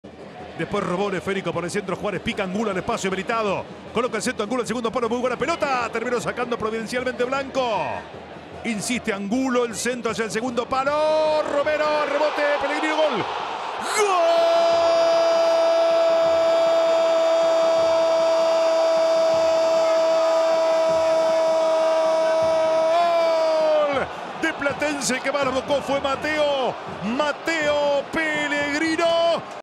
Golazo de Mateo Pellegrino🔥 ¡Revive este momento clave que hizo vibrar a los hinchas! ⚫️⚪️
Golazo de MateoPellegrino para sellar la victoria de Platense sobre Boca! ⚽️🔥 ¡Revive este momento clave que hizo vibrar a los hinchas! 👏⚫️⚪️